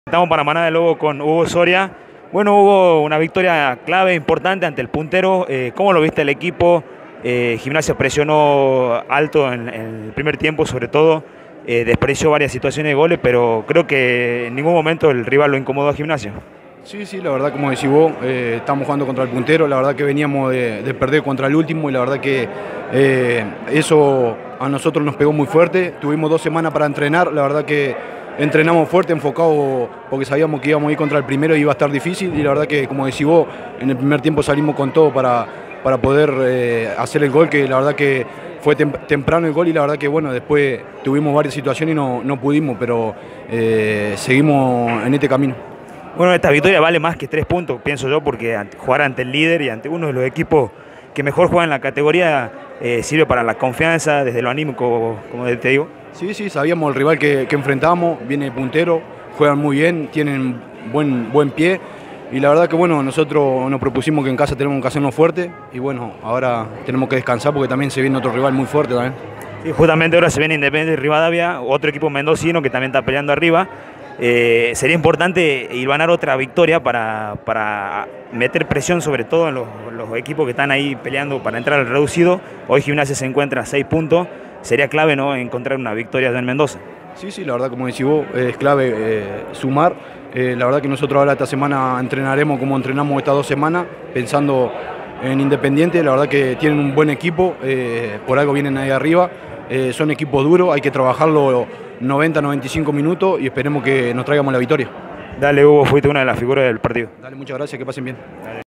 FECHA 26 | GIMNASIA 1 DEP MAIPU 0 | HUGO SORIA
Post triunfo, dialogamos con Hugo Soria, una de las claves en el mediocampo para quedarse con los puntos en casa.